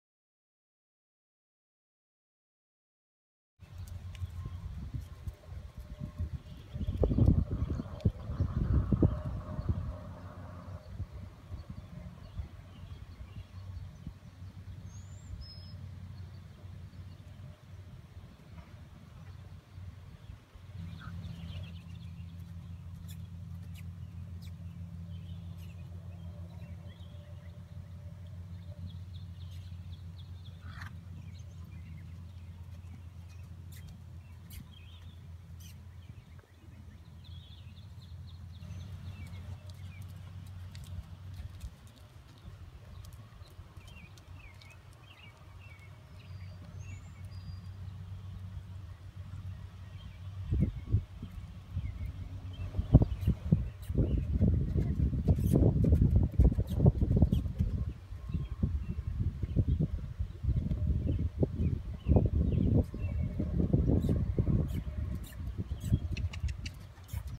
This Toots Is Hot.... Peggy Combo
Hip #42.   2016 Sorrel Filly by Extremely Hot Chips x Tootsee Roll.  Peggy is a Tom Powers LL and Stakes Sale Entry